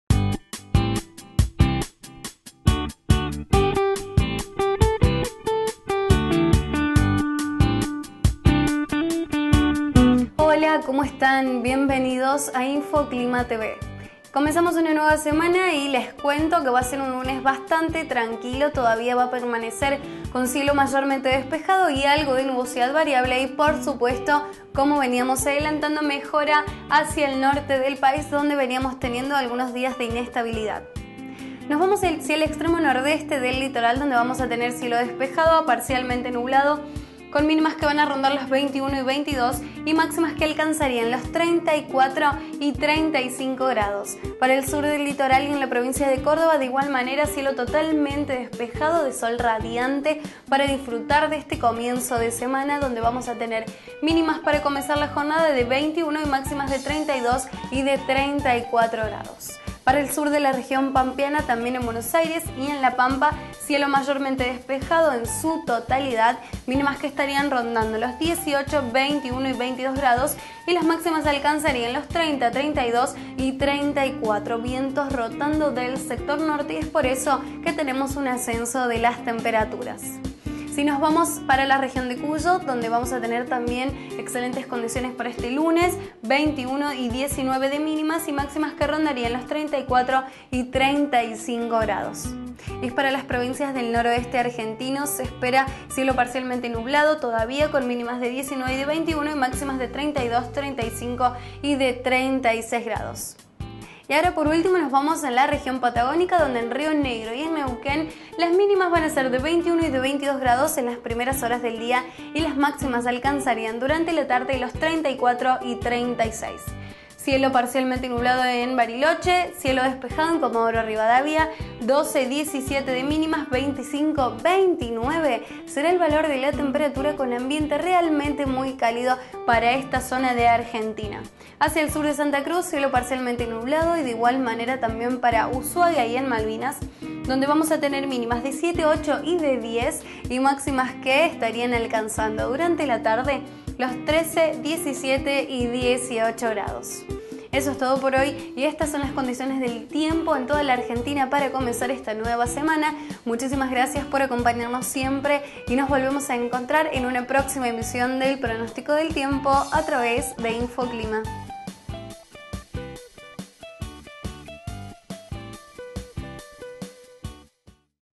Pronóstico para el 5 de enero de 2015 Argentina - Infoclima TV.
Pronóstico del tiempo elaborado por Infoclima para toda la República Argentina. 5 de enero de 2015 - Presenta: Sofía Garone.